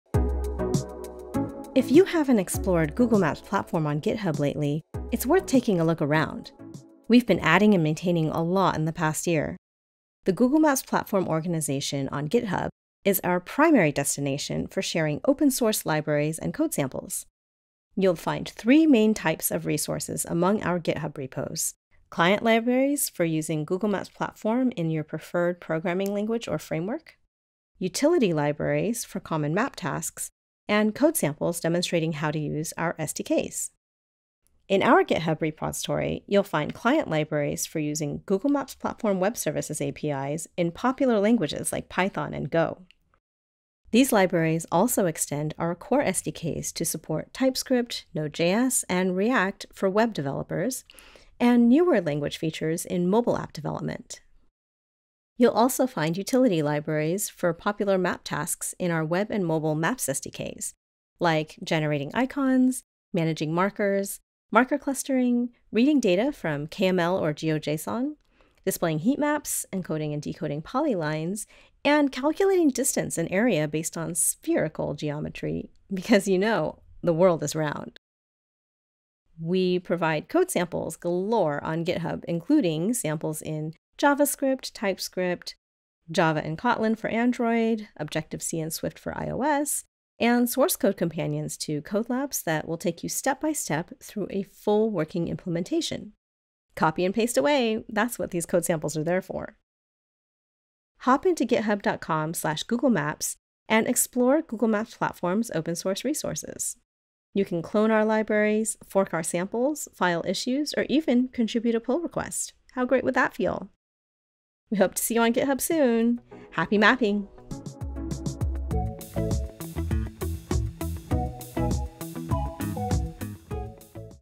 Clone these GitHub repos from Google Maps Platform
Looking for utility libraries for common map tasks, code samples demonstrating how to use our SDKs, or client libraries in a specific programming language? Get a tour of the 50+ open-source repositories maintained by the Google Maps Platform team on GitHub.

product: Maps Platform - General;